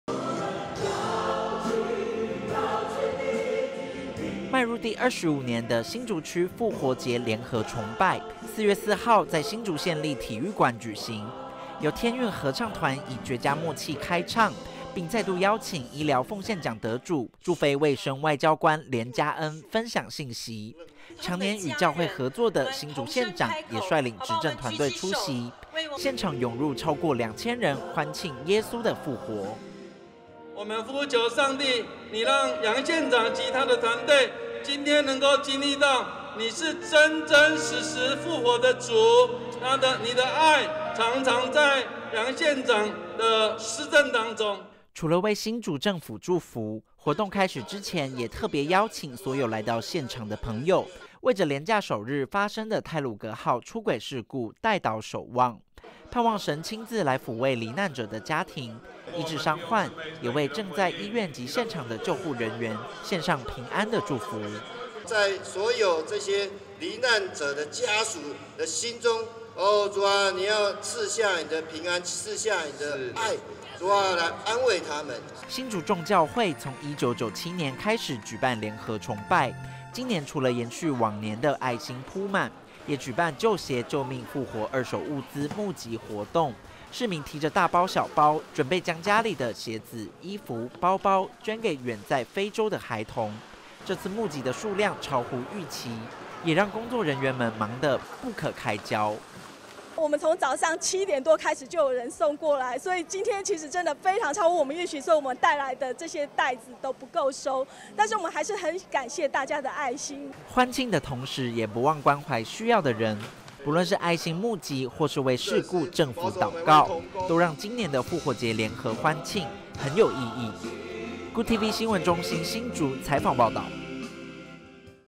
新竹復活聯合崇拜25年 聯合關懷傳遞愛與溫暖-GOOD TV NEWS
基督教華人教會新聞盡在GOOD TV

邁入第25年的新竹區復活節聯合崇拜4/4在新竹縣立體育館舉辦，由天韻合唱團以絕佳默契開唱，並再度邀請醫療貢獻獎得主，駐非衛生外交官連加恩分享信息，長年與教會合作的新竹縣長也率領執政團隊出席，現場湧入超過兩千人歡慶耶穌的復活。

除了為新竹政府祝福，活動開始之前也特別邀請所有來到現場的朋友，為著連假首日發生的太魯閣號出軌事故代禱守望，盼望神親自來撫慰罹難者的家庭，醫治傷患，也為正在醫院及現場的救難人員獻上平安的祝福。

新竹眾教會從1997年開始舉辦聯合崇拜，今年除了延續往年的愛心撲滿，也舉辦「舊鞋救命復活二手物資」募集活動，市民提著大包小包，準備將家裡的鞋子、衣服、包包捐給遠在非洲的兒童。這次募集的數量超乎預期，也讓工作人員們忙得不可開交。

歡慶的同時也不忘關懷需要的人。不論是愛心募集或是為事故、政府禱告，都讓今年的復活節聯合歡慶，很有意義。 GOOD TV新聞中心新竹採訪報導